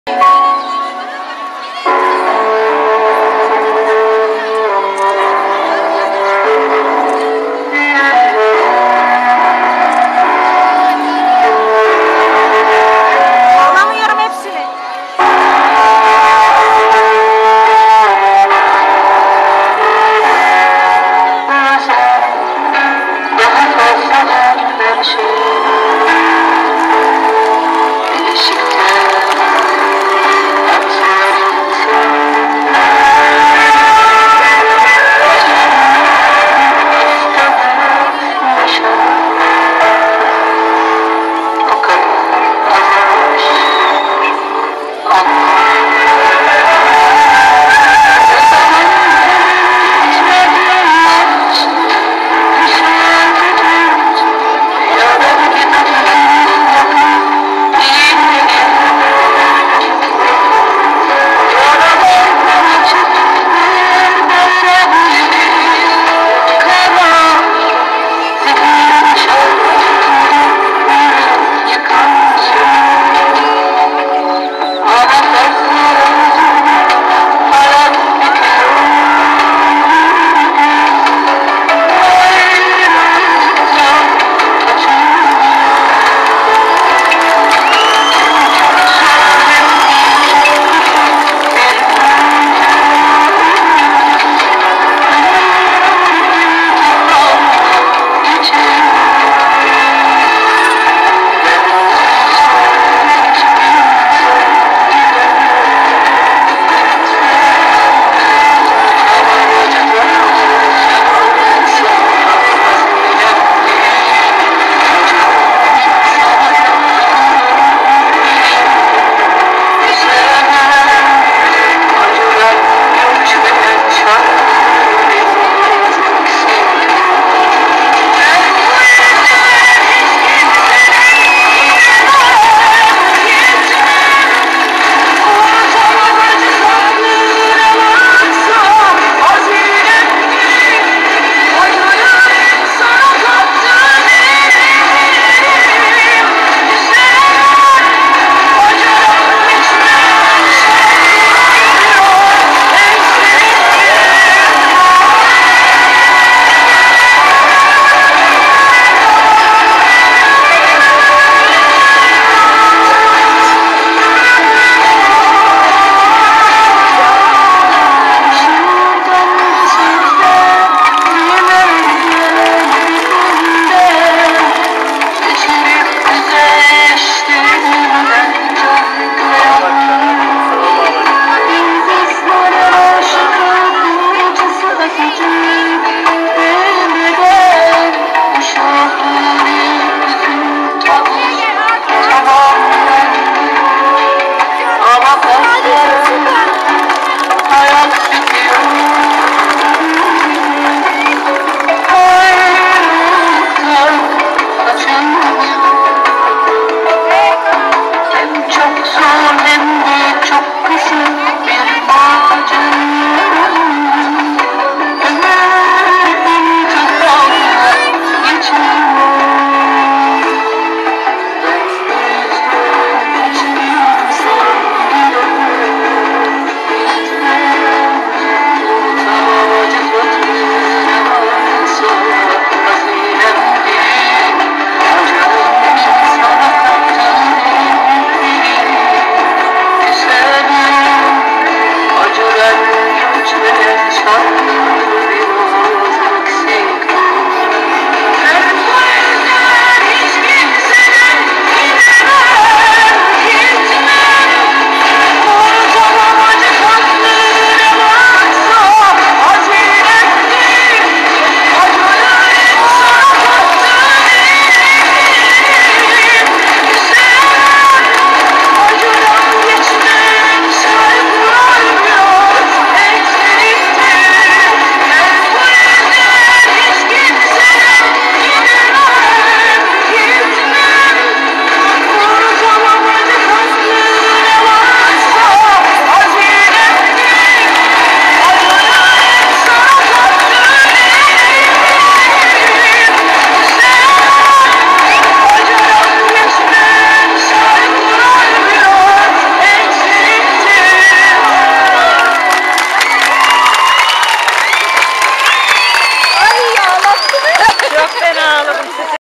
cafer tayyar i.ö.o.23 nisan gösterileri
anasınıfı sabahçı gurubu 23 nisan gösterileri